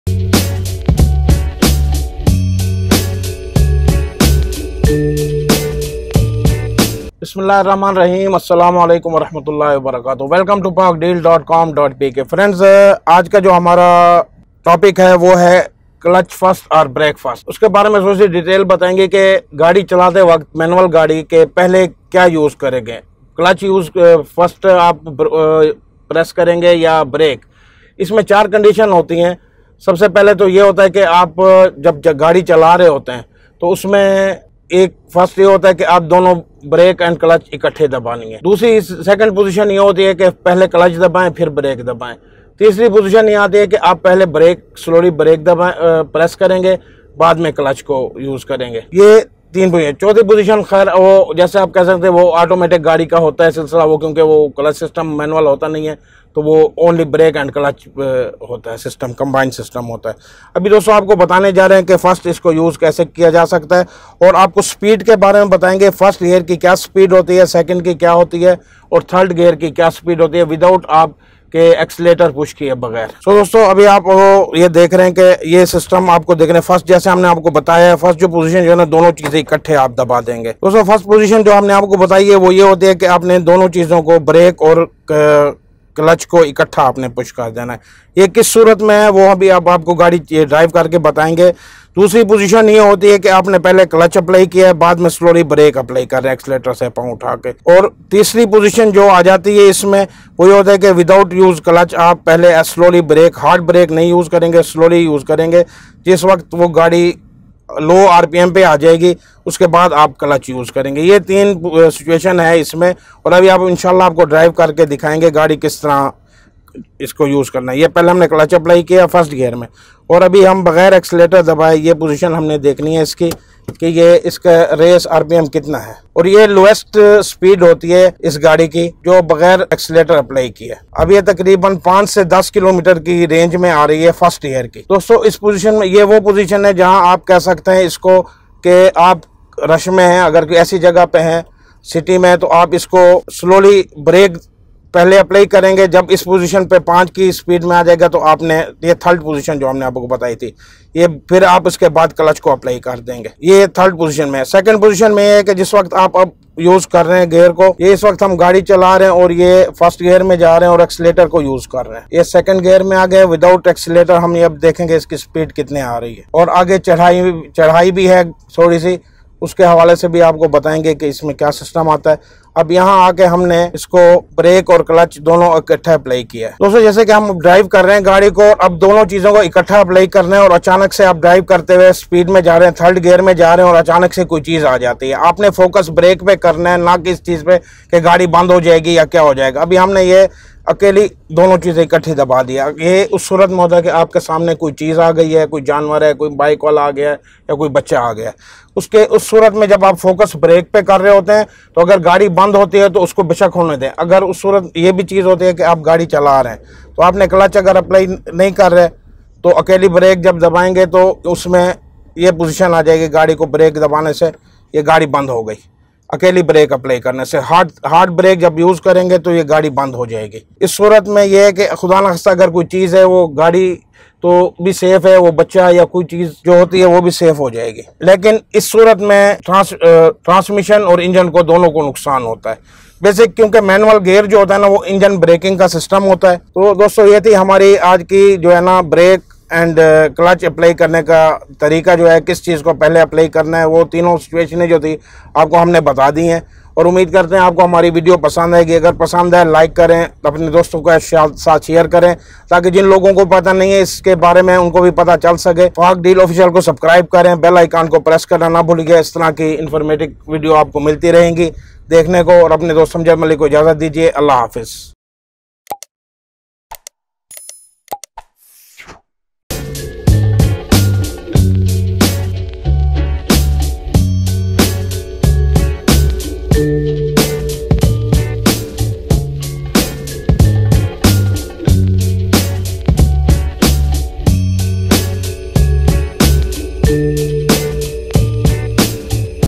How To Drive A Manual Car - Manual Driving Tips
How To Drive A Manual Car - Pak Deal Official

Typically, manual cars will have three pedals: clutch, brake, and accelerator (in that order, left to right).

The brake and accelerator pedals are pretty simple - press the brake to slow down (the firmer you press it, the quicker you’ll lose speed), while using the accelerator increases the engine’s revs and causes you to speed up (the firmer you press it, the quicker you will speed up).

The clutch pedal is the one that makes driving a manual car more difficult than an auto.

Without going into too much detail, the clutch is essentially two metal plates that connect the engine to the drive wheels.

So by pressing down on the clutch pedal, you’re disconnecting the engine from the wheels.